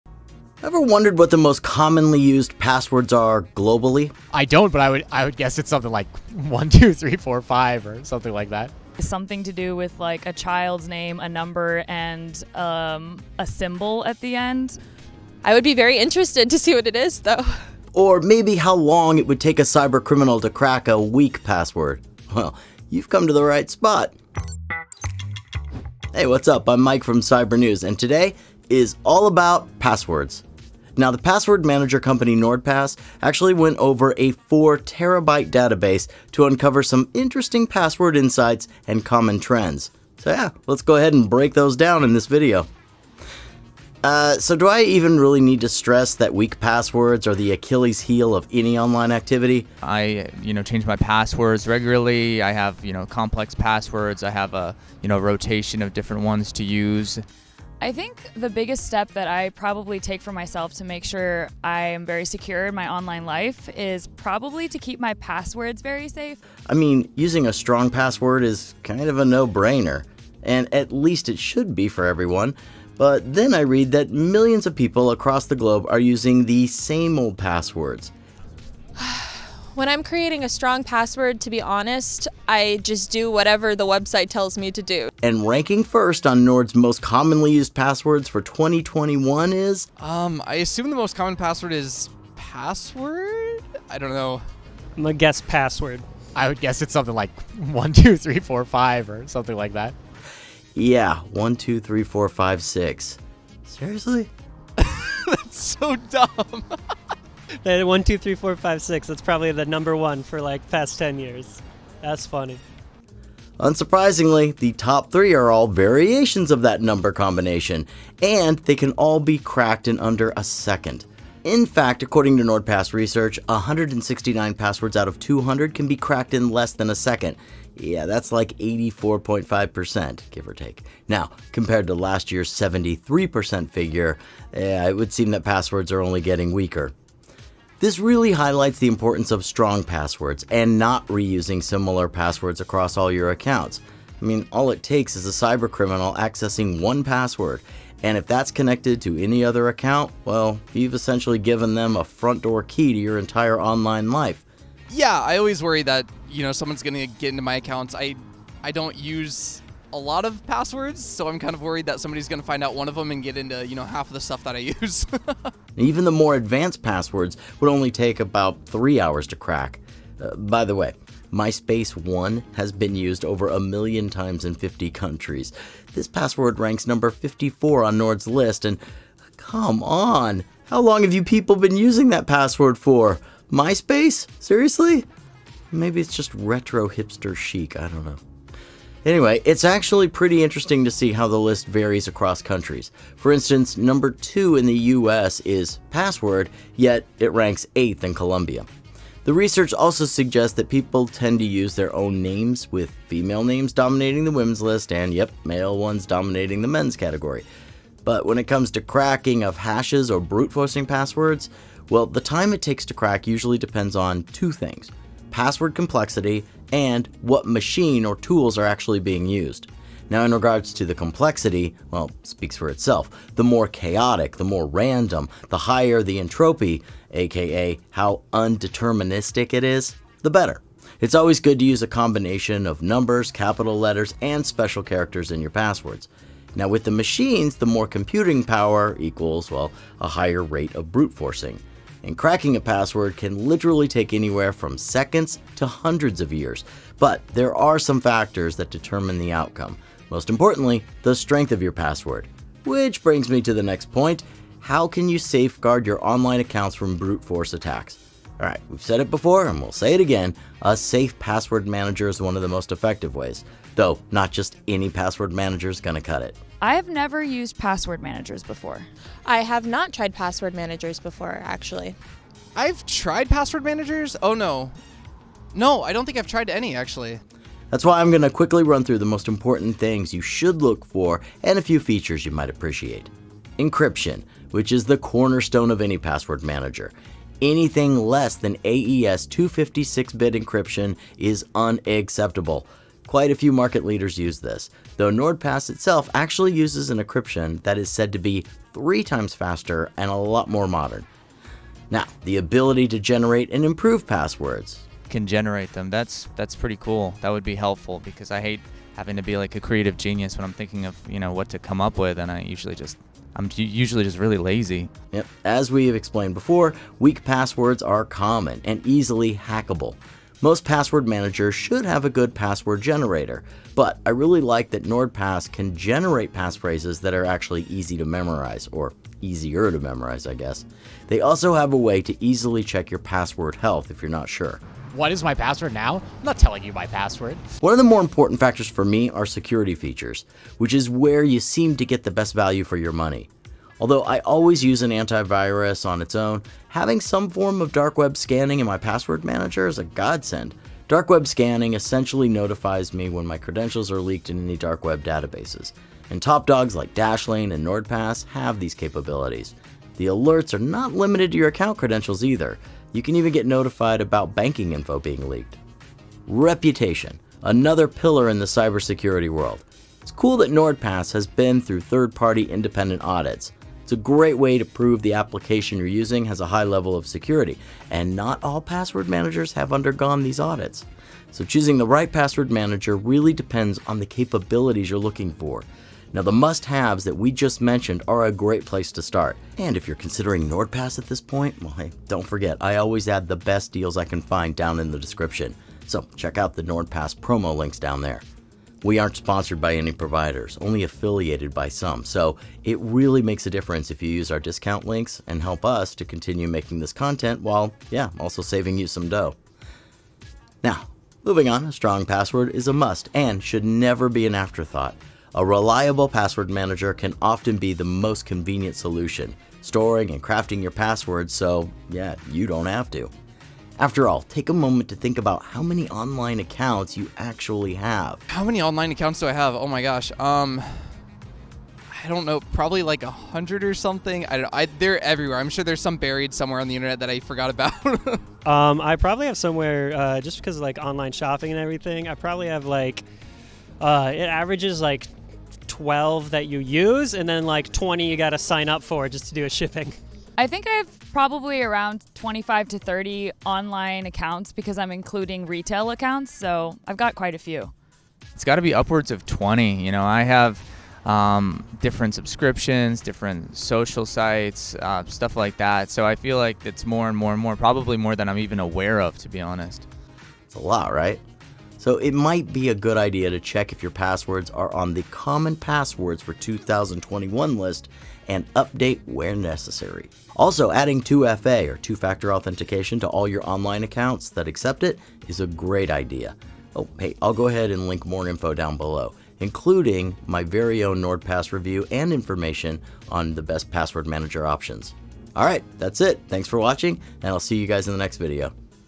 NordPass: the most common passwords in the world
🔥  Ensure your password security NOW 🔥

Ever wondered what the most common passwords are, globally? More interestingly, how long would it take a cybercriminal to crack a weak password? Well, the password manager company, NordPass went over a 4TB database, in order to offer some interesting password insights, and common trends, so let’s break it down in this video…

Now, I don’t need to stress that weak passwords are the achilles heel of your online lives.

🔑  Using a strong password is a no brainer...at least it should be. That’s what I thought, until I read that millions of people across the globe have, or are using the same ol’ passwords. Ranking 1st on Nord’s “Most Commonly Used Password 2021”list is ‘123456.’ Unsurprisingly, the top 3 are all variations of this number combination. AND, can all be cracked in under 1 second!

💪  This really highlights the importance not only of strong passwords, but not reusing similar passwords across your accounts. All it takes is a cybercriminal accessing 1 password, and if that’s connected to another account, you’ve essentially given them a front door key to your online life.

⁉️ How to safeguard your online lives from brute force attacks:

 I’ve said this before, and I’m going to say it again, a safe password manager is one of the most effective ways. Though, not just any password manager will cut it.

That’s why I’ll just quickly run through the most important things you should look for and a few features I always expect...

💯  Encryption, being the cornerstone of any password manager. Anything less that AES 256-bit cipher is unacceptable. NordPass itself actually uses an encryption that is said to be three times faster, and a lot more modern. In short, this is end-to-end encryption and even the staff at NordPass can’t access your login details.

💯  The ability to generate and improve passwords! Again, key feature. As we explained, weak passwords are common and easily “hackable.”  Most password managers will have this, at the very least. Though, I really like that NordPass 2022  can generate phrases that are easier to memorize. They also have a way to easily check your password health, if you’re not sure.

💯  Okay, arguably, one of the most important factors for me are security features. This is where you seem to get the best value from the money you’ve actually spent. Dark web scanning essentially notifies you if your credentials are leaked in any dark web databases. Top dogs like Dashlane password manager and NordPass have such capabilities. The alerts are not limited to your account credentials, so you can even get notified about any banking info being leaked.

💯  Reputation, again, is another pillar in the cybersecurity world. It’s cool that NordPass has been through 3rd-party, independent audits. This is a great way to prove the application you’re using has a high level of security, and not all password managers have undergone such audits.

➡️  Final thoughts: A strong password is a must, and should never just be an afterthought - a reliable password manager can often be the most convenient solution; storing and crafting your passwords so - you don’t have to. Afterall, take a moment to think about how many online accounts you actually have…

00:00 Intro
00:25 Common password research
03:28  How passwords are cracked
04:14 How a reliable password manager can protect users
08:00 Final thoughts